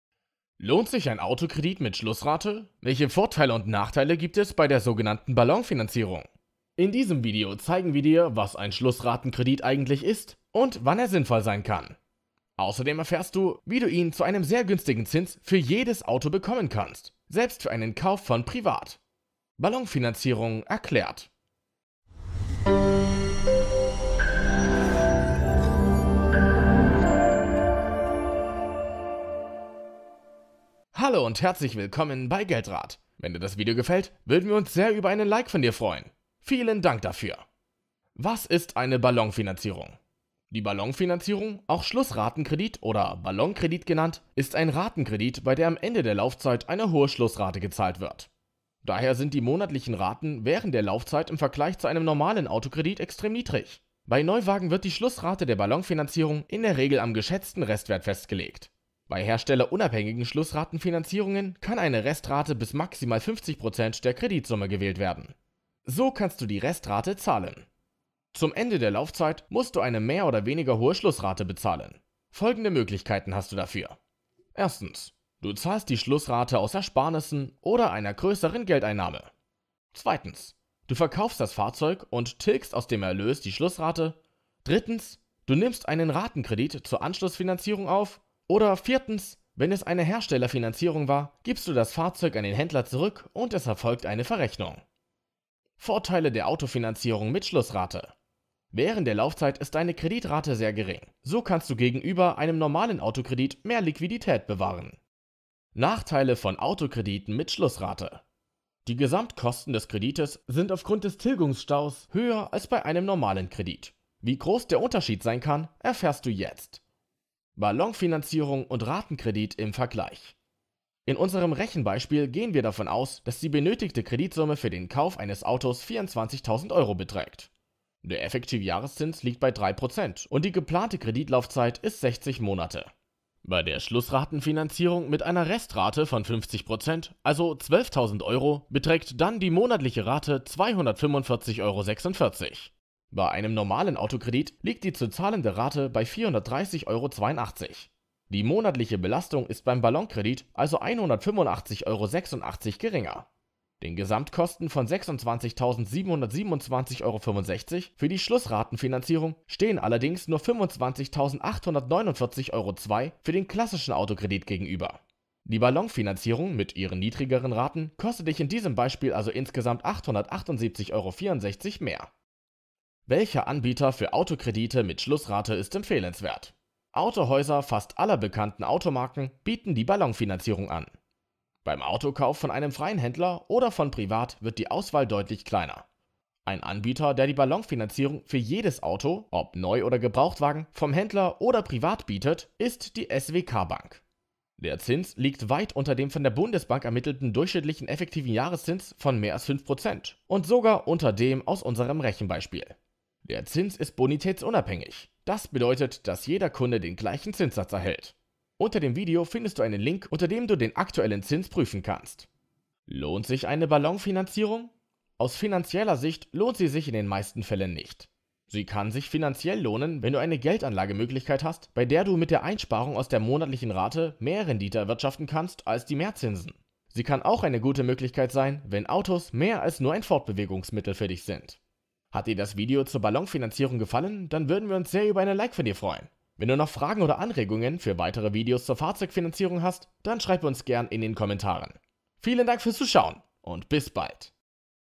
Ballonfinanzierung - Lohnt sich der Autokredit mit Schlussrate?
Die SWK Bank bietet keine Ballonfinanzierung mehr.

Die Vorteile und Nachteile der Ballonfinanzierung

In diesem Video beantworten wir die folgenden Fragen:
Was ist eine Ballonfinanzierung? Schlussratenfinanzierung erklärt-.
Welche Ballonkredit Anbieter gibt es für Gebrauchtwagen?
Dauer einer Ballonfinanzierung?
Für wen lohnt sich ein Autokredit mit Schlussrate?
Welcher Anbieter ist günstig?
Wie kann man die Restrate bezahlen
Kann man das Auto zurückgeben?
Wie kann man ein mit Ballonkredit finanziertes Auto verkaufen?

Ausserdem zeigen wir Dir ein Beispiel mit Berechnung der Raten.

Inhalt:
0:38 Was ist eine Ballonfinanzierung?
1:05 Möglichkeiten für die Restrate
1:35 Vorteile der Autofinanzierung mit Schlussrate
1:45 Nachteile Autokredite mit Schlussrate
1:57 Ballonfinanzierung und Ratenkredit im Vergleich
2:53 Empfehlenswerte Anbieter
3:32 Lohnt sich eine Ballonfinanzierung?

Die Videos dienen lediglich zu Informationszwecken und sind keine Anlageberatung!